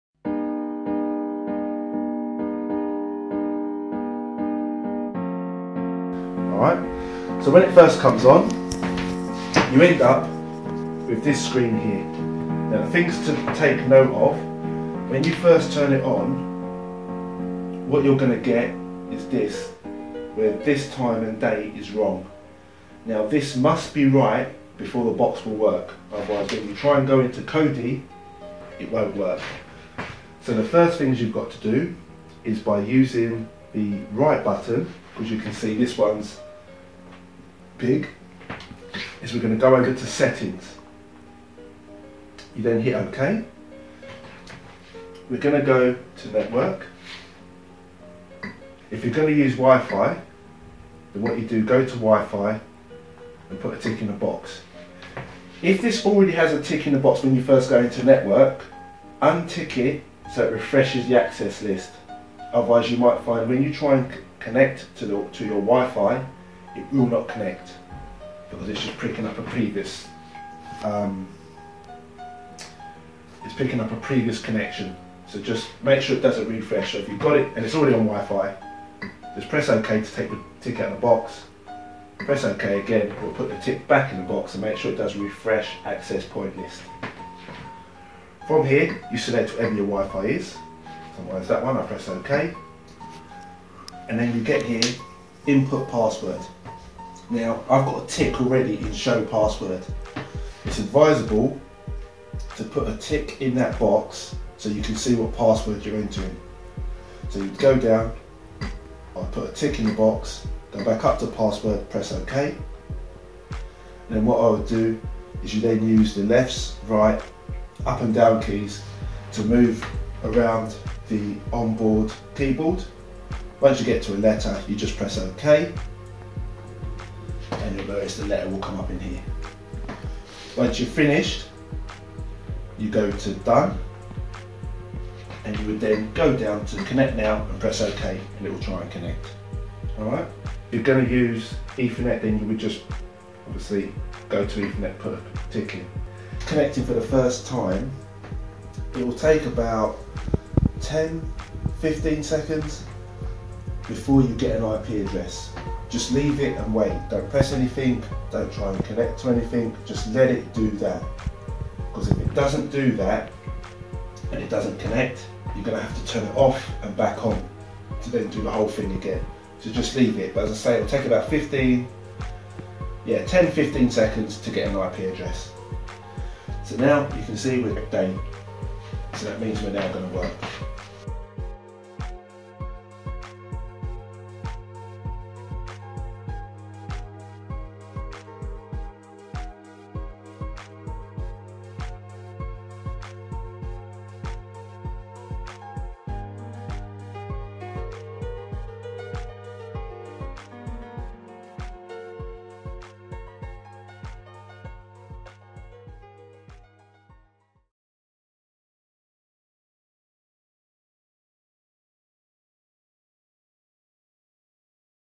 Connecting Android MKV TV Box to the internet
How to set the Android box up to the internet.  Make sure that the time is correct before you try and launch any apps otherwise they will not work properly.